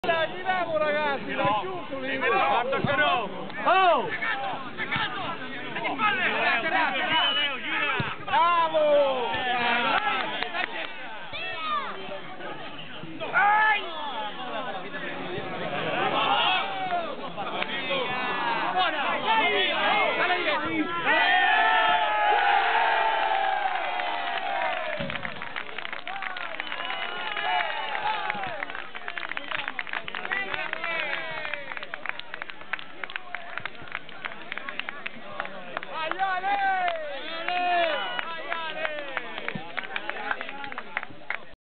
7a) BACCI.MOV
23/10/2010 7a Giornata Campionato Prima Categoria: SAN MARCELLO vs LAURENTINA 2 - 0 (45° Gol di Bacci)